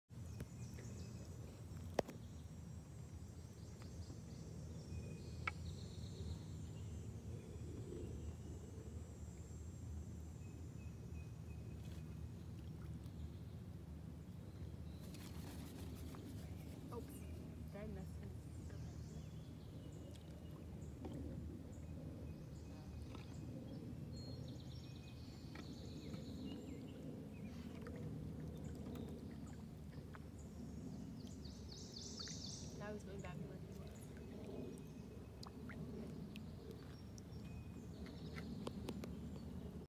Great Blue Heron on St Croix River
Great blue heron on the St Croix River near William O'Brien State Park in MN